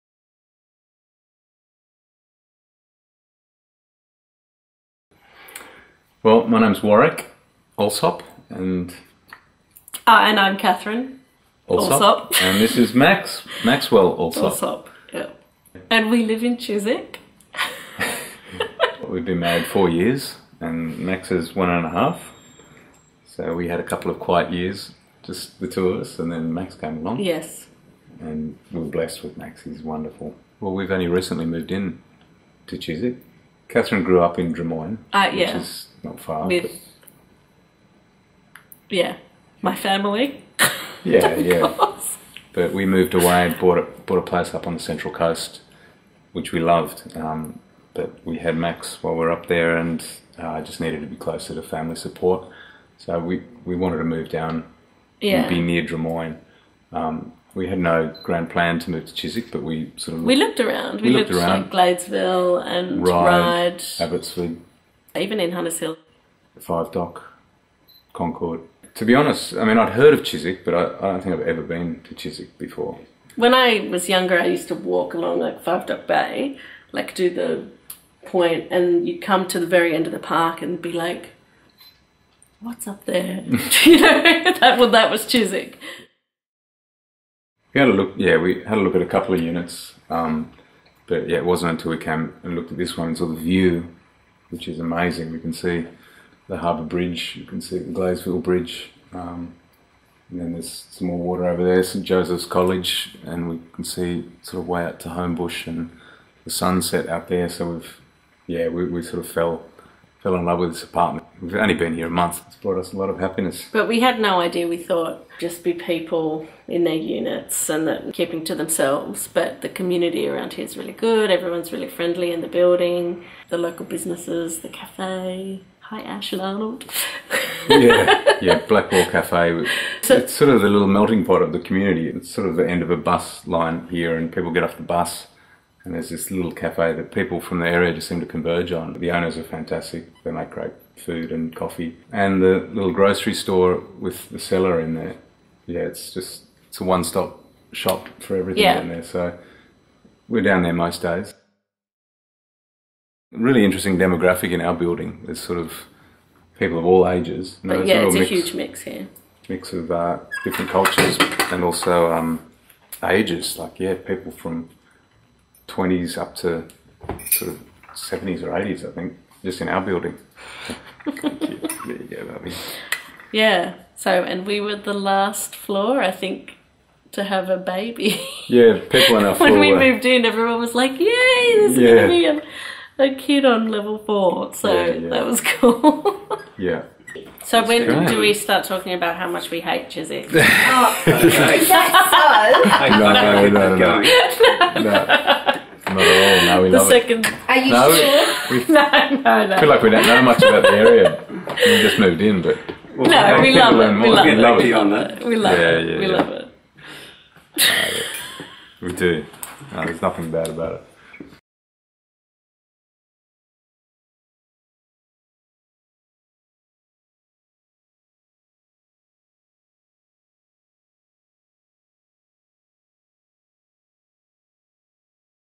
CHISWICK: CHANGES AND CONNECTIONS - Katherine, Warwick and Maxwell Allsopp
CHISWICK: CHANGES AND CONNECTIONS, a multimedia history and community stories project promoting a sense of place in the riverside suburb of Chiswick, Sydney, Australia. Featuring Katherine, Warwick and Maxwell Allsopp, this clip forms part of a series of 16 short films consisting of oral histories and impressions of living in Chiswick, as recounted by both old and new residents. Produced in 2015 for the City of Canada Bay by Particles / It’s a Long Story. Supported by The NSW Government Family and Community Services and Local Government NSW.